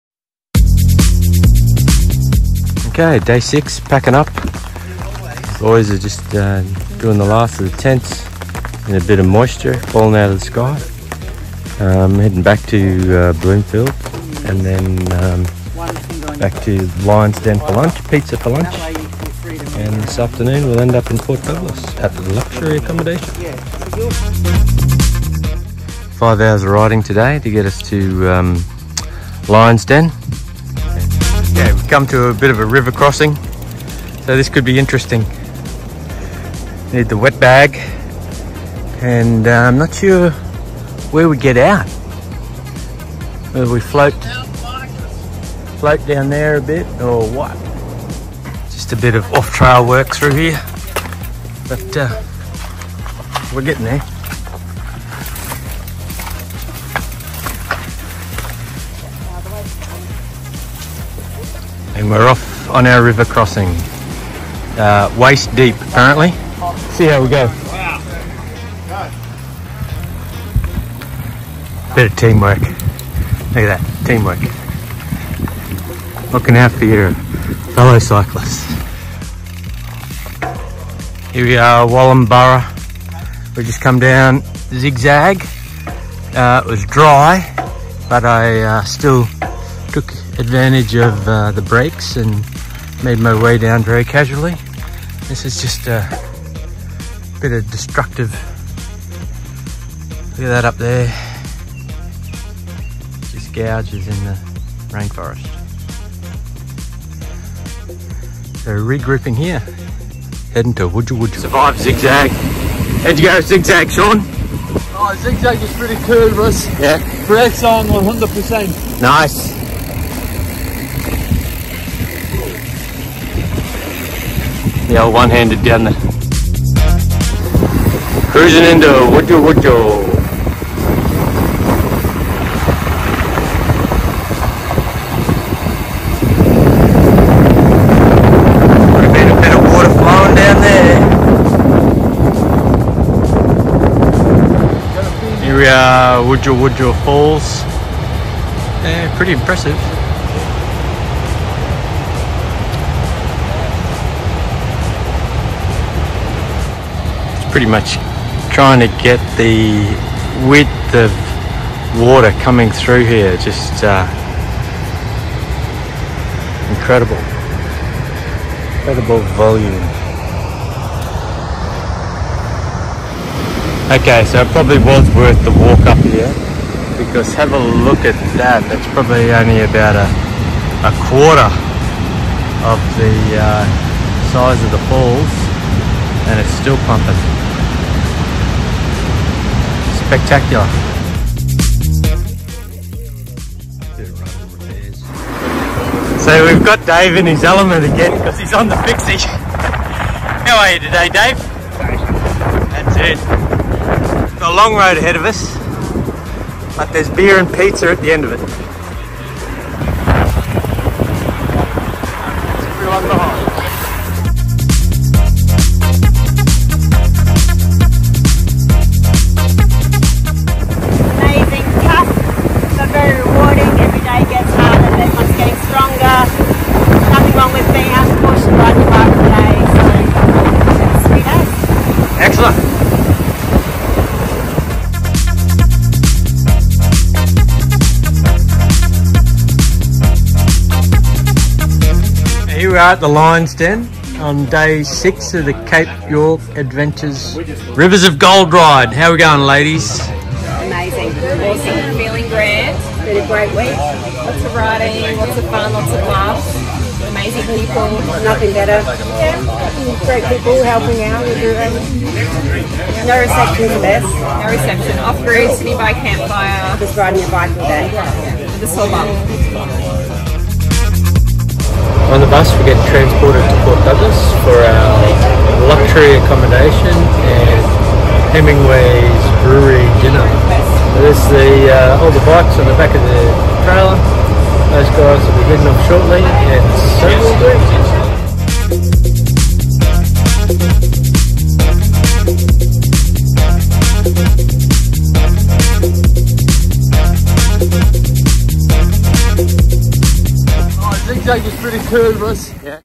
2024 Cape York Mountain Bike Adventures - Rivers Of Gold Day 6
Day 6: 66km China Camp to Port Douglas

A little creek crossing before a ride down the infamous Zig Zag into Wujal Wujal to see the Falls then lunch at the Lion's Den Hotel. Comfortable transfer to Port Douglas for beers at Hemingway’s.